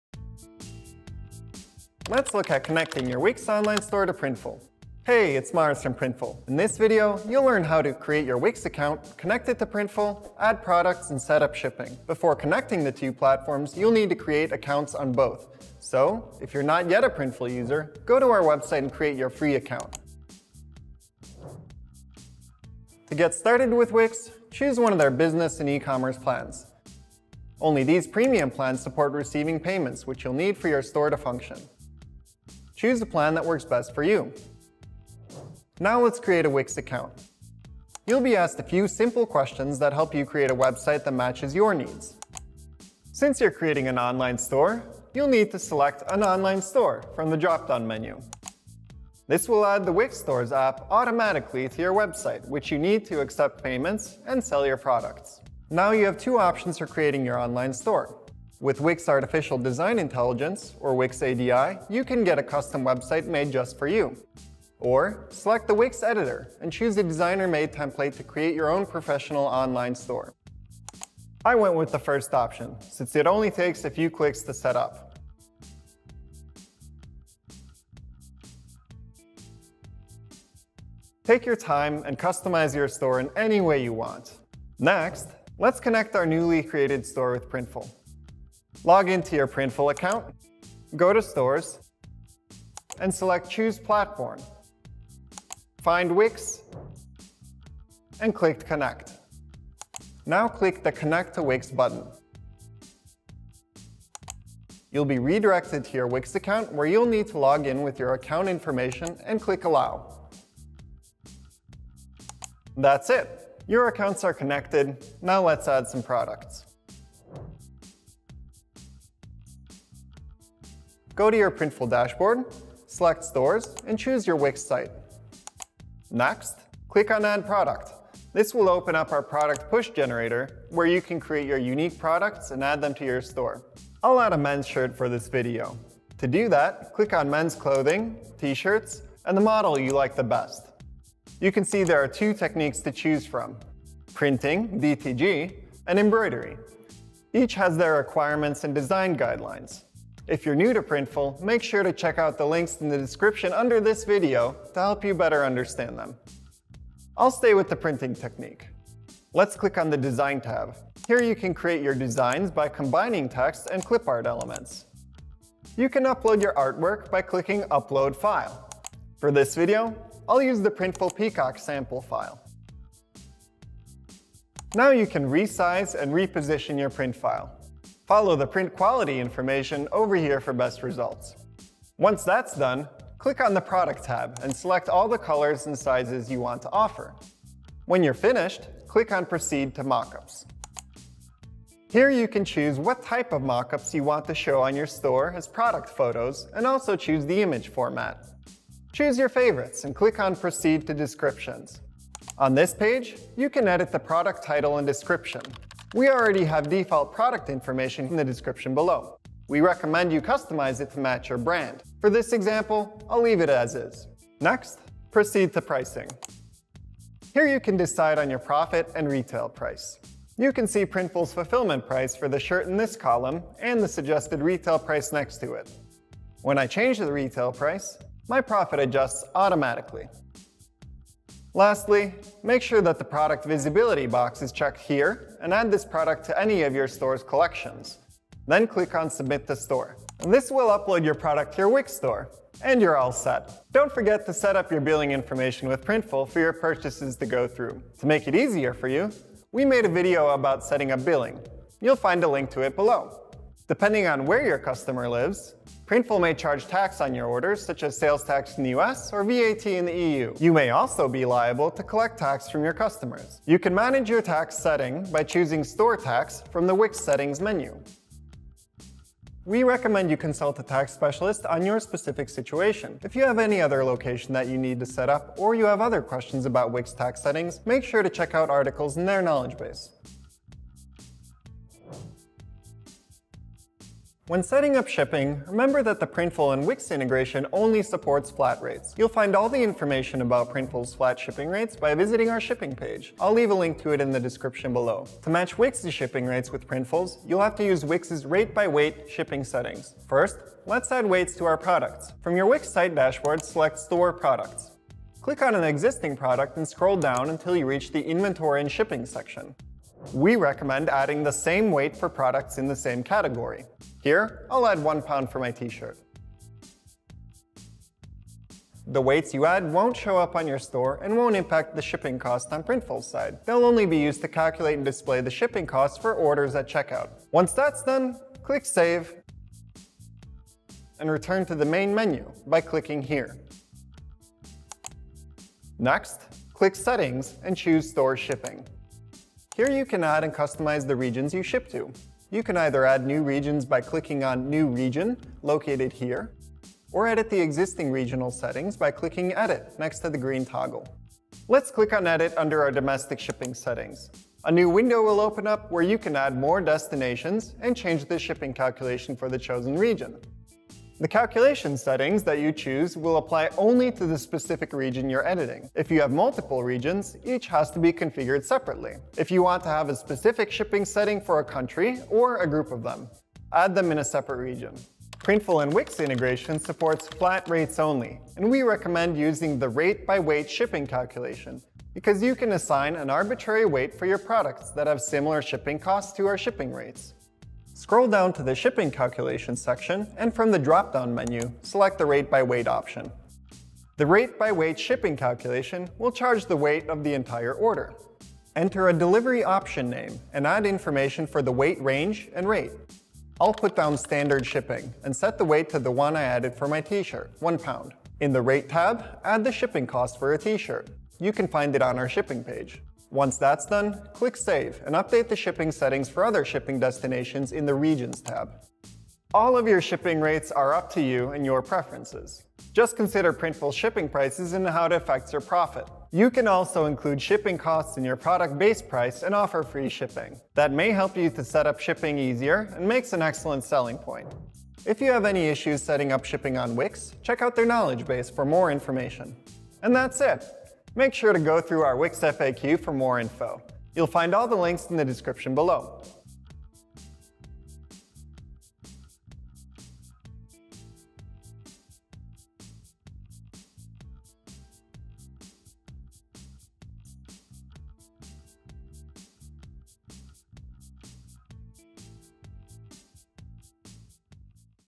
How to Integrate with Wix - Printful 2019
Over 160 million people have a Wix site and can now sell custom-designed Printful goods there. Explore Wix's many features, from 500+ customizable and beautiful designer-made storefront templates to 250+ free apps to kickstart your online store!

Monday to Friday from 9am to 8pm EST